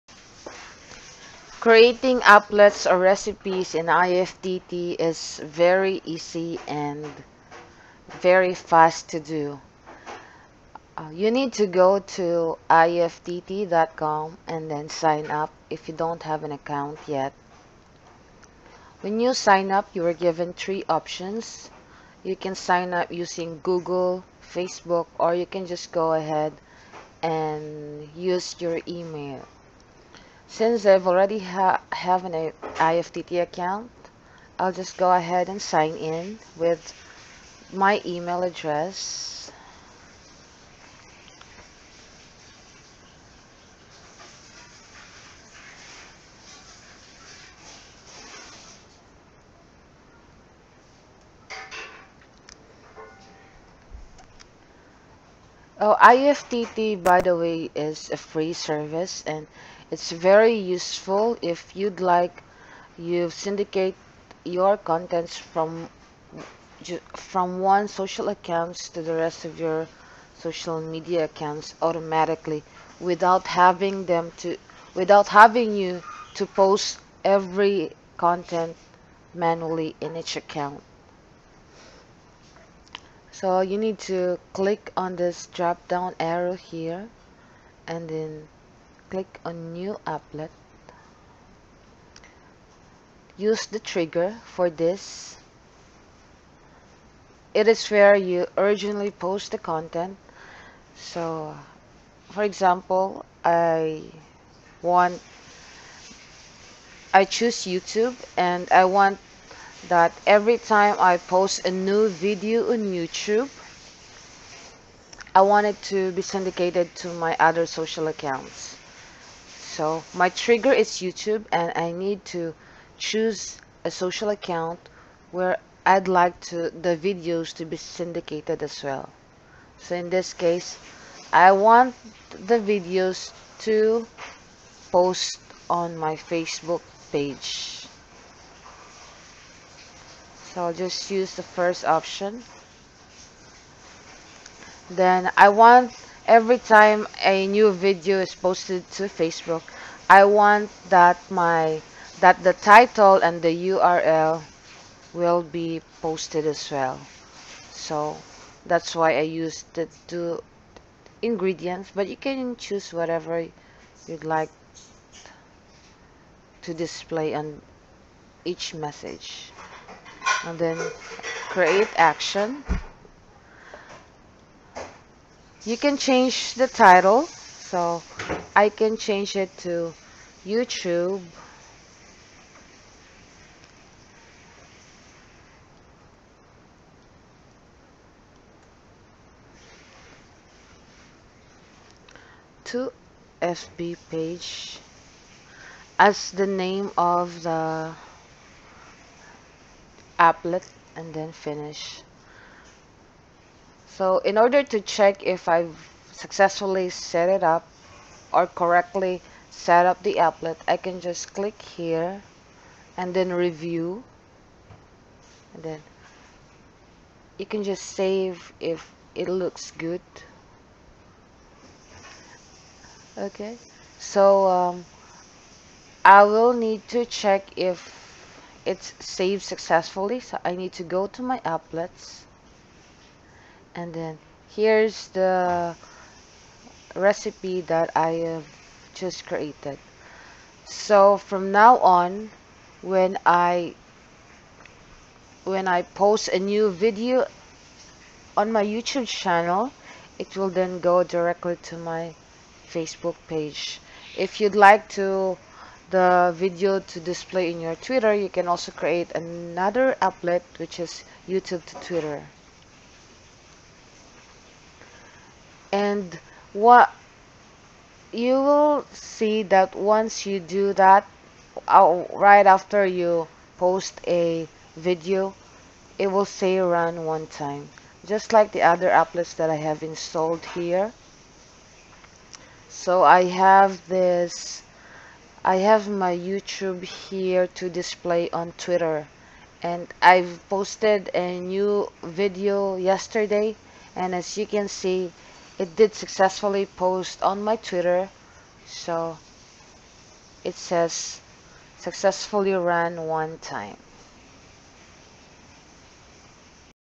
How To Create IFTTT Recipe or Applet
Creating ifttt applets/recipes is fast and easy, follow these steps:

2. Click the dropdown arrow at the top right side beside the username
3. Click 'New Applet'
4. Click 'This' choose your social media trigger (i.e. Youtube)
5. Choose the 2nd option (your preference)
6. Click 'That', any account where you'd like to post to syndicate to (i.e. Facebook Page)
7. Choose option one (your preference)
8. Create applet